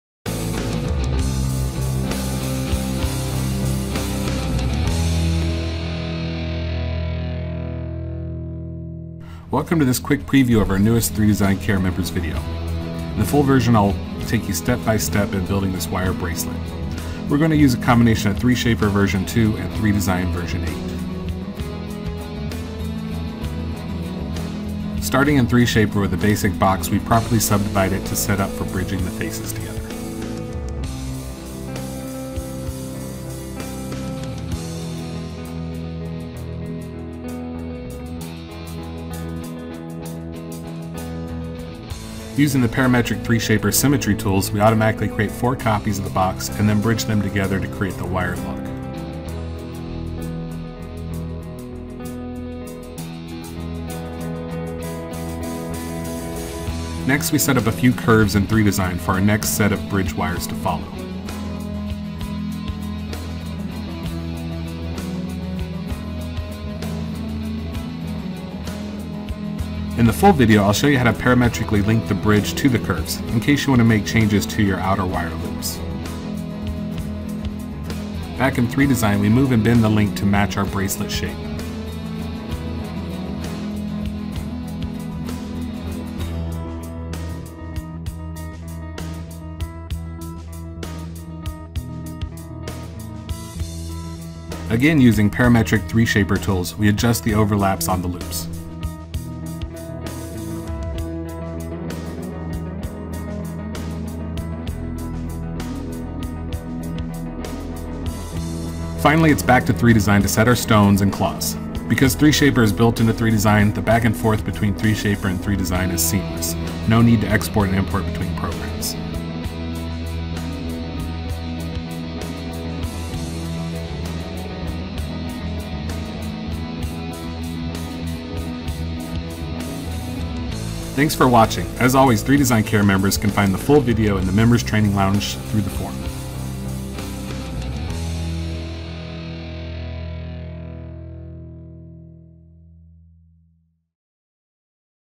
Preview Wire Bracelet in 3DesignCAD8
In this project, we build a  parametric wire bracelet using a combination of 3Shaper & 3DESIGN tools to create a custom setting and bracelet link.
The full video can be found on the member's lounge section of the 3DESIGN forum.